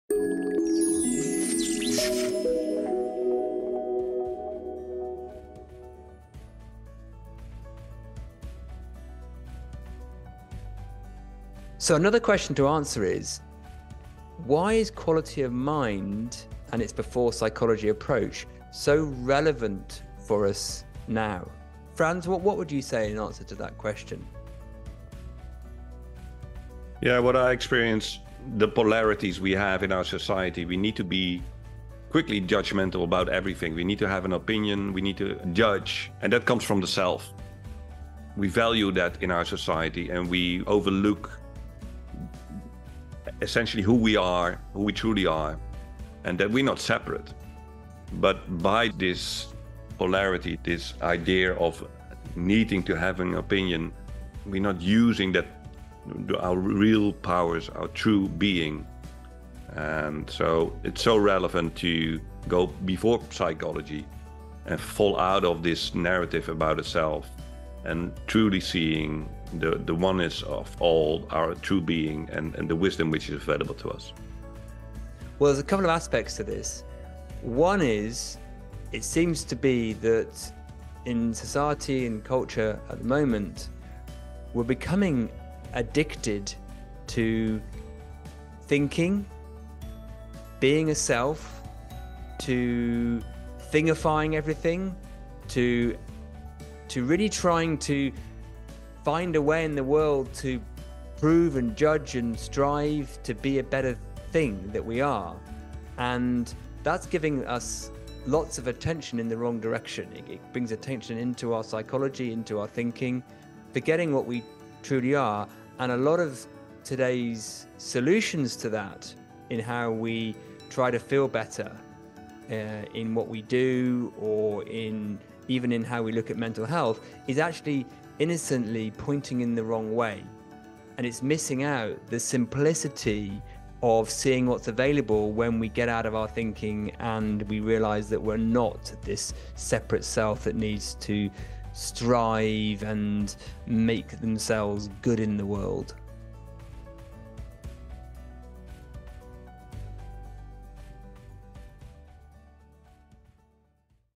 ''Why is Before Psychology So Relevant for Today's World?''
Quality of Mind Open Programme:  ''Why is Before Psychology so relevant for Today's World? ''

Quality of Mind: The Before Psychology approach to peace, performance and potential - get curious and explore more via our Podcast Reach out with any questions.

0:07 FAQ Intro
0:25 Answer 1
1:14 Answer 2
2:26 More Info